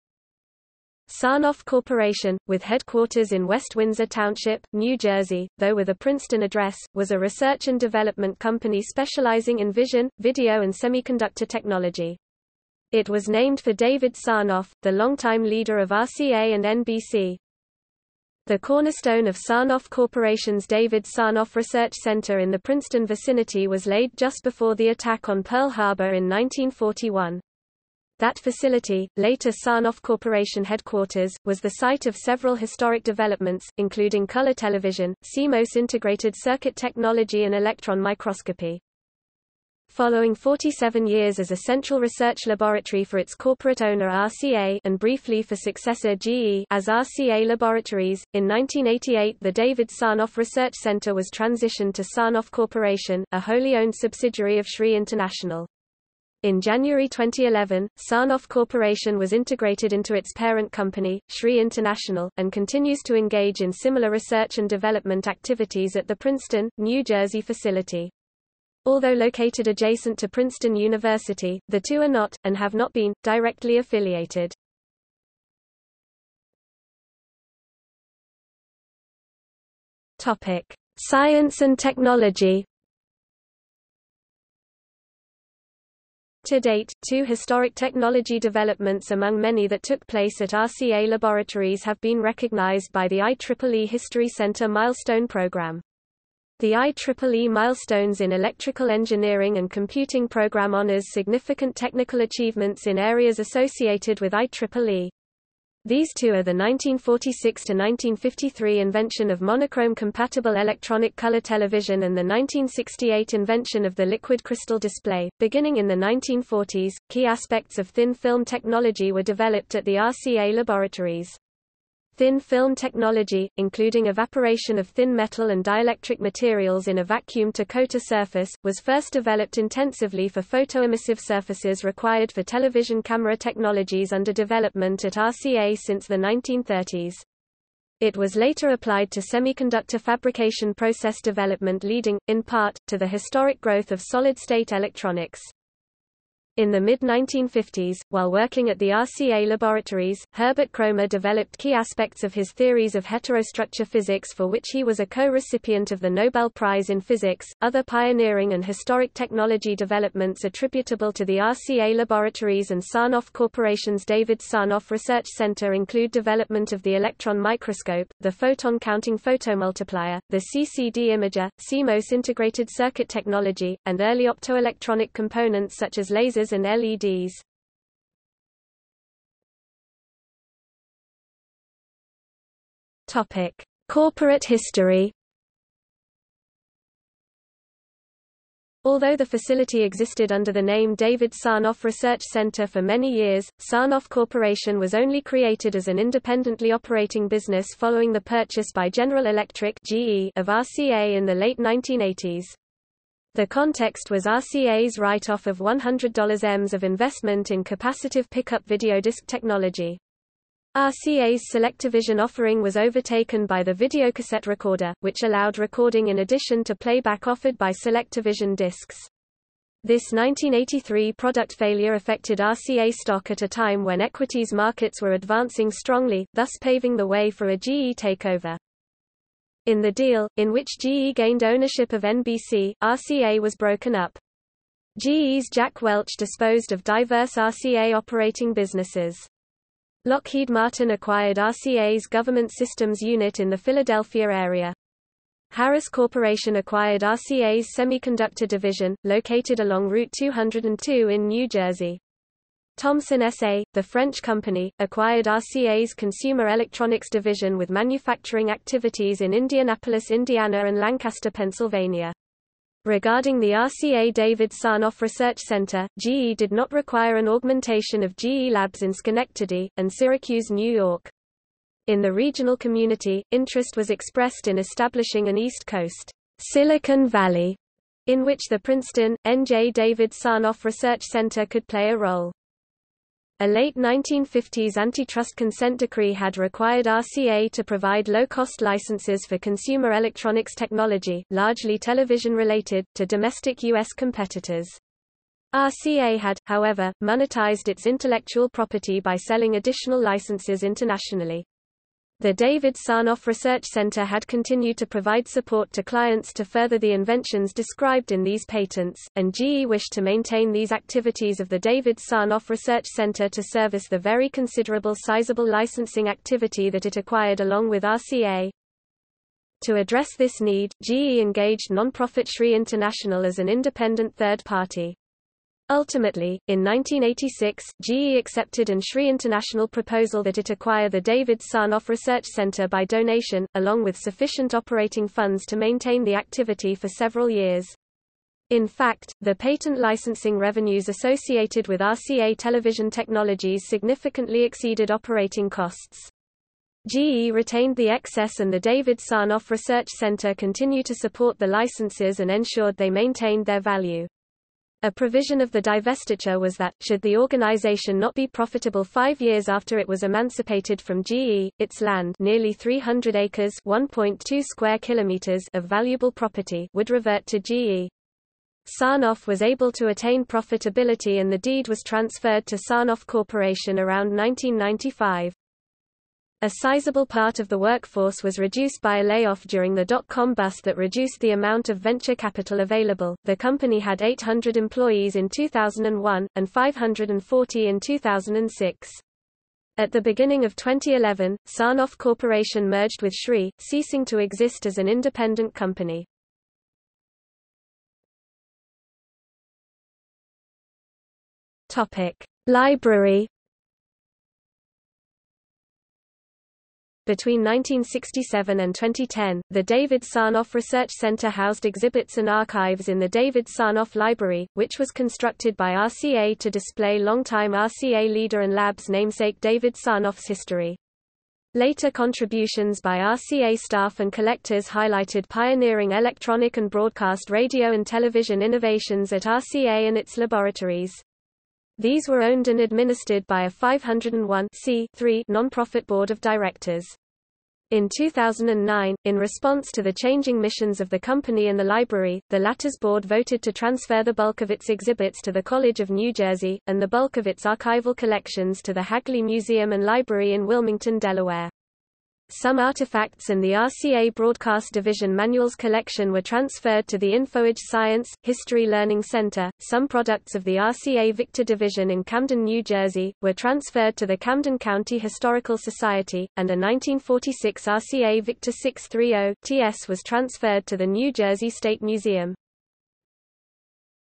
RCA Laboratories | Wikipedia audio article
00:01:16 1 Science and technology
00:02:59 2 Corporate history
00:06:38 3 Library

- Socrates

SUMMARY
Sarnoff Corporation, with headquarters in West Windsor Township, New Jersey, though with a Princeton address, was a research and development company specializing in vision, video and semiconductor technology. It was named for David Sarnoff, the longtime leader of RCA and NBC.
The cornerstone of Sarnoff Corporation's David Sarnoff Research Center in the Princeton vicinity was laid just before the attack on Pearl Harbor in 1941.  That facility, later Sarnoff Corporation headquarters, was the site of several historic developments, including color television, CMOS integrated circuit technology and electron microscopy.
Following 47 years as a central research laboratory for its corporate owner RCA (and briefly for successor GE) as RCA Laboratories, in 1988 the David Sarnoff Research Center was transitioned to Sarnoff Corporation, a wholly owned subsidiary of SRI International. In January 2011, Sarnoff Corporation was integrated into its parent company, SRI International, and continues to engage in similar research and development activities at the Princeton, New Jersey facility.